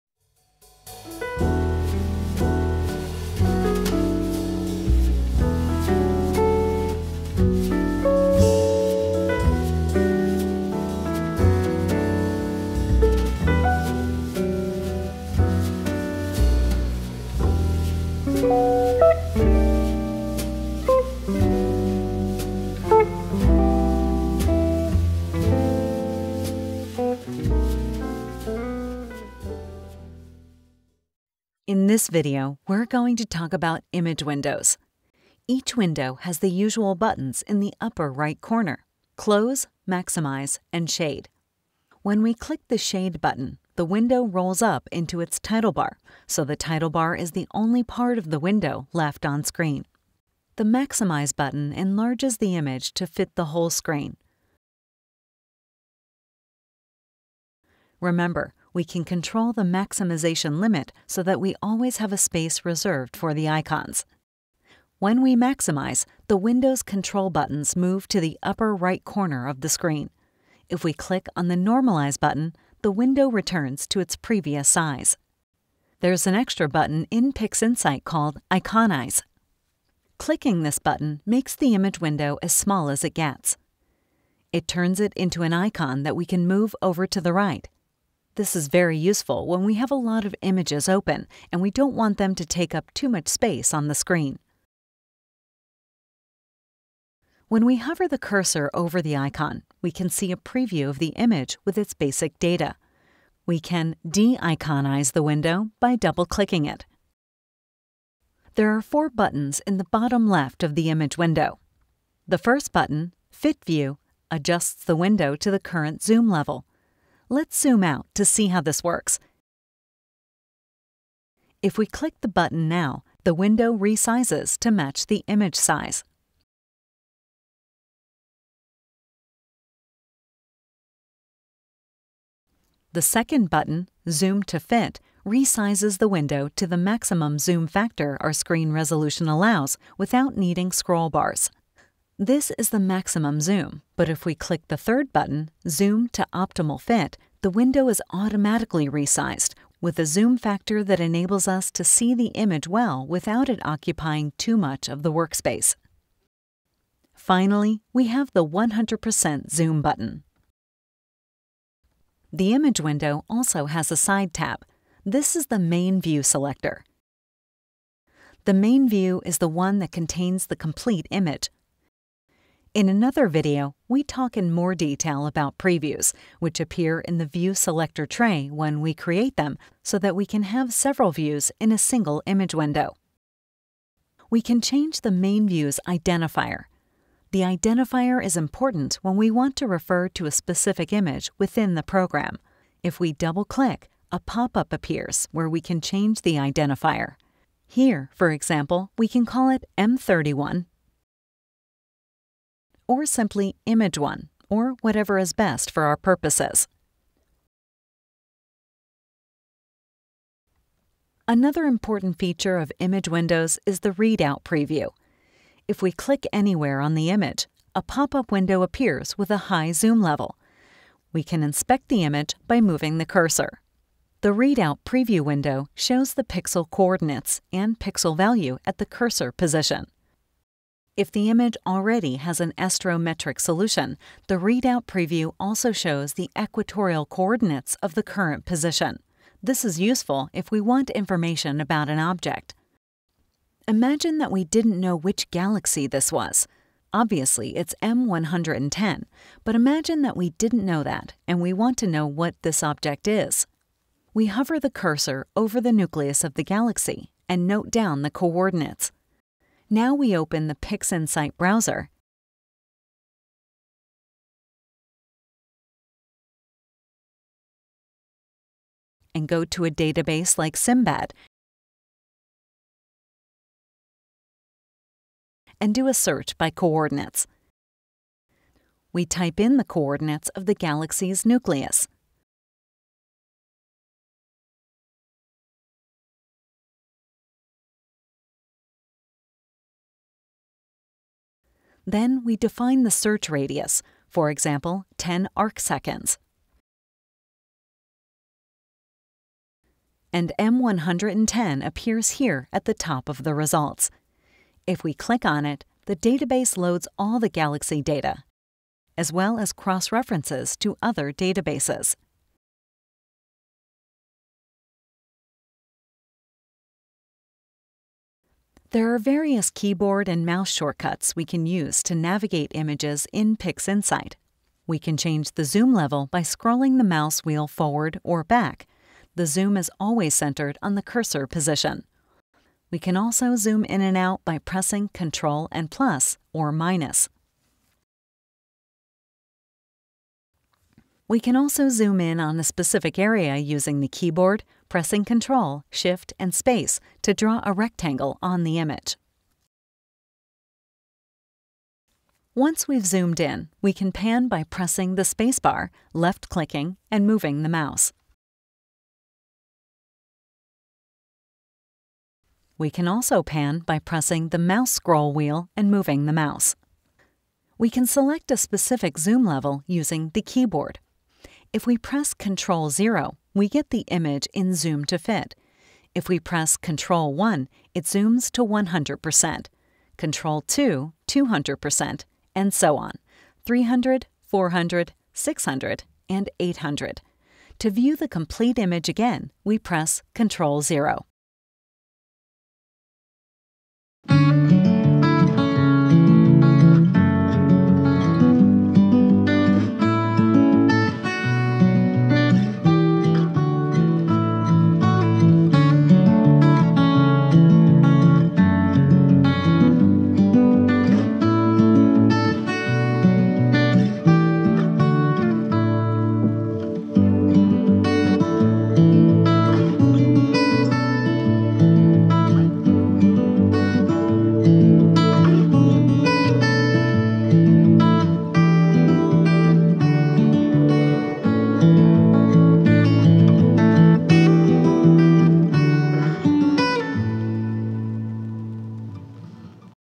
Introduction to PixInsight — 04 Image Windows
Introduction to PixInsight image windows

00:31 Image window control buttons
02:41 Main view selector
03:29 Reading out pixel values and celestial coordinates
05:16 Image navigation keyboard and mouse shortcuts